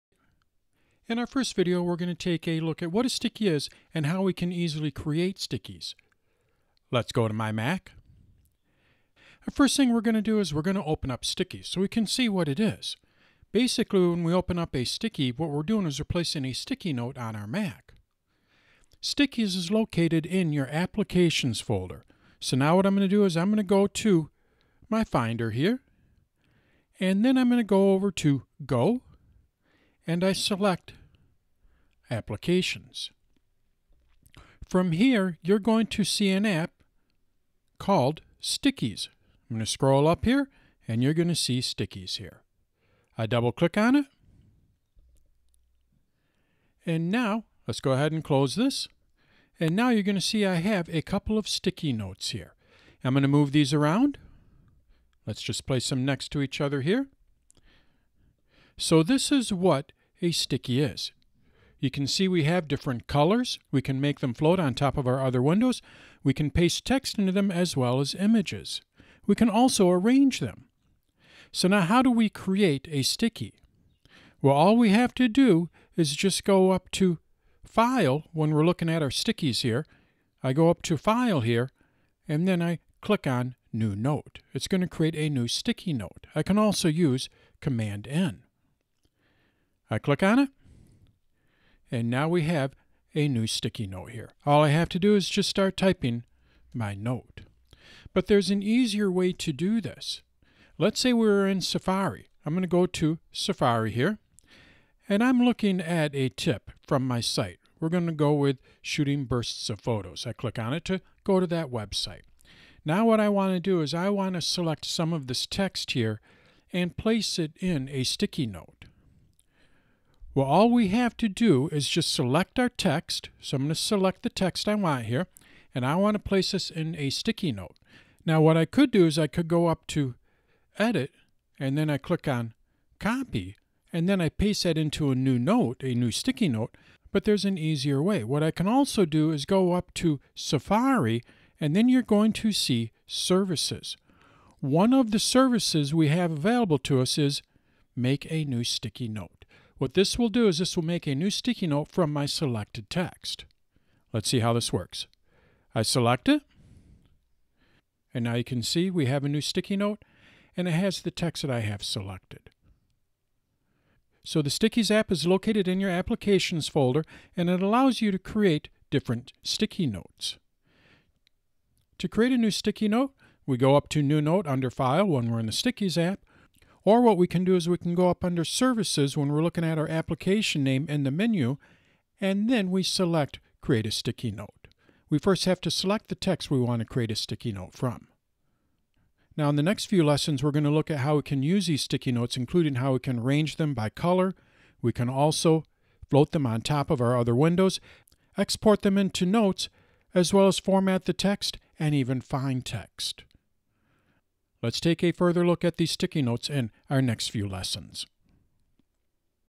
What is Stickies and Creating Stickies
In this lesson for Stickies on the Mac we look at what the Stickies app is and how we create Stickies notes.